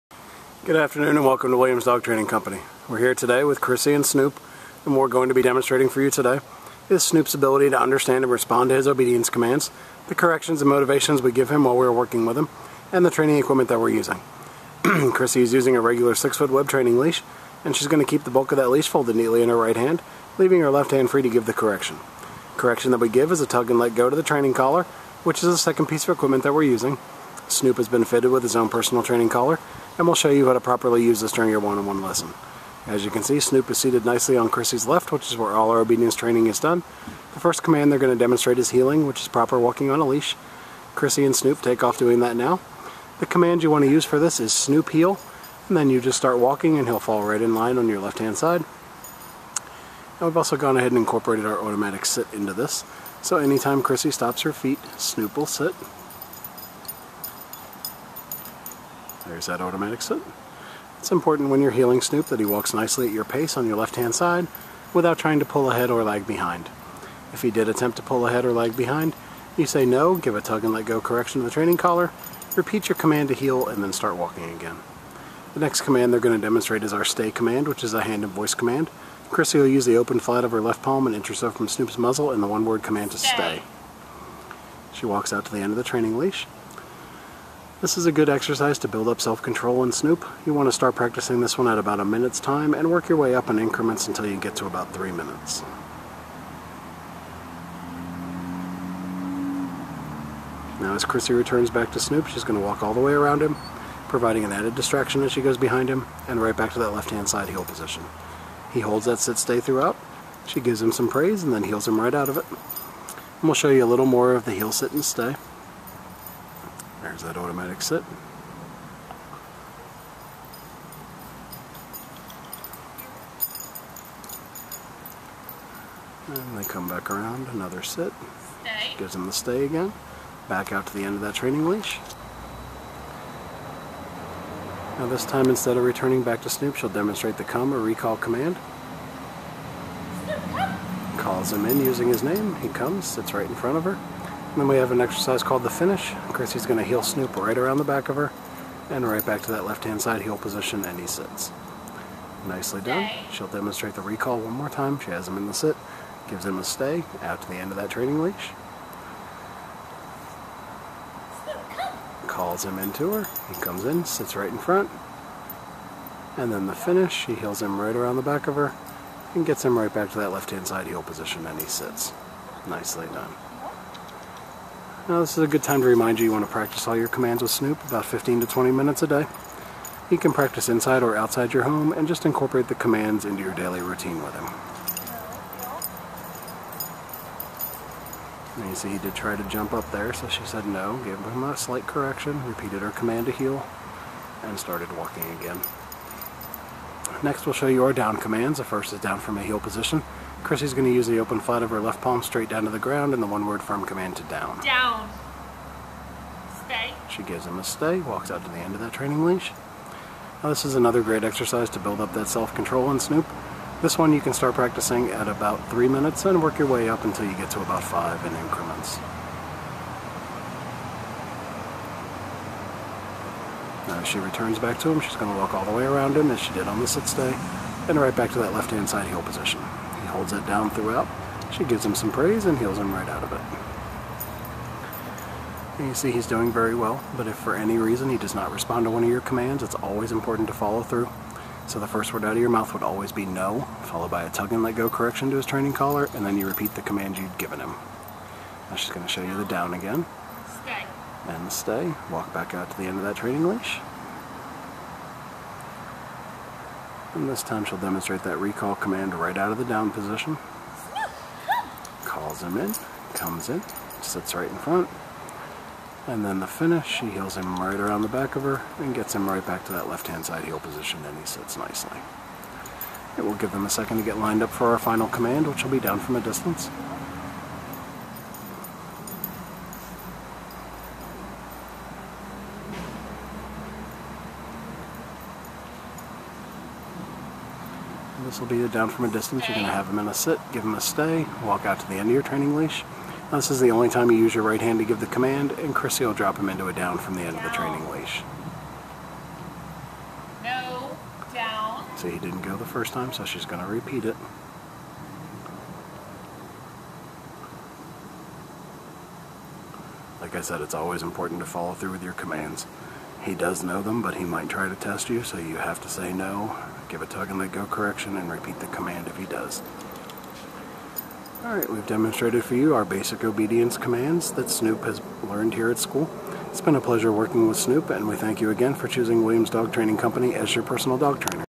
Snoop.mp4
Snoop the puggle mix performs the obedience commands he learned here at Williams Dog Training Company